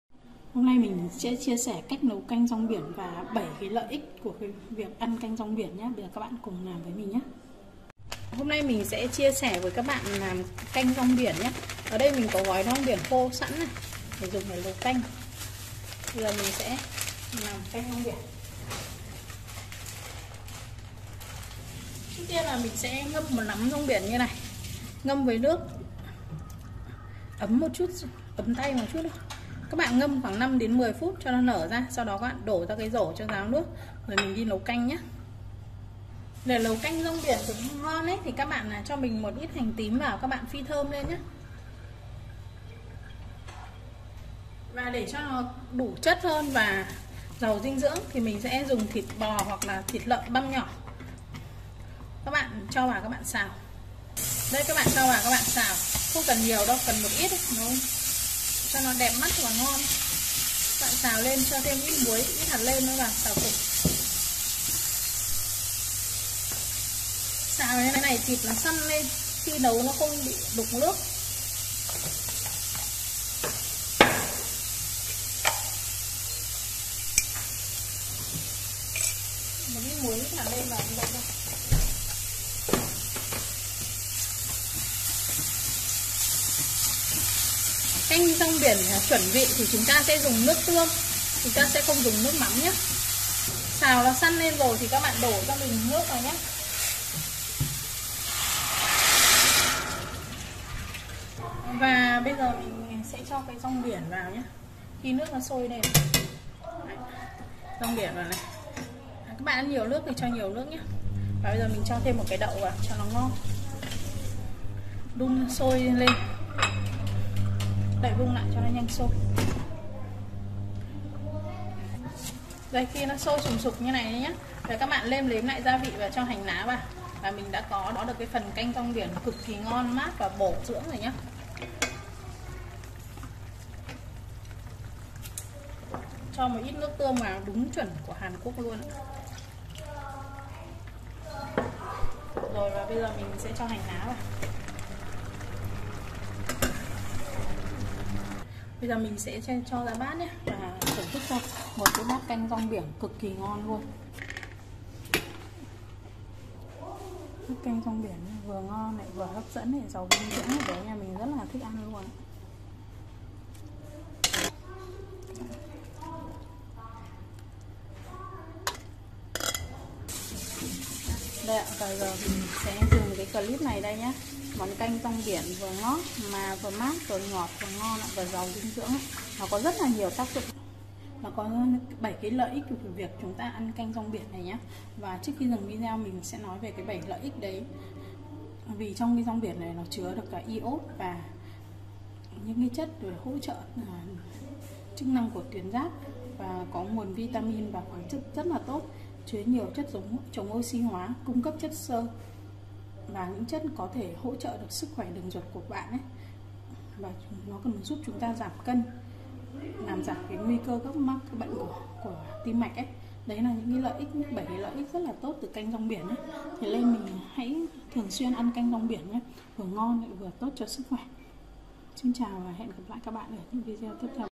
CÁCH NẤU CANH RONG BIỂN KHÔNG TANH
Ăn canh rong biển rất tốt cho sức khỏe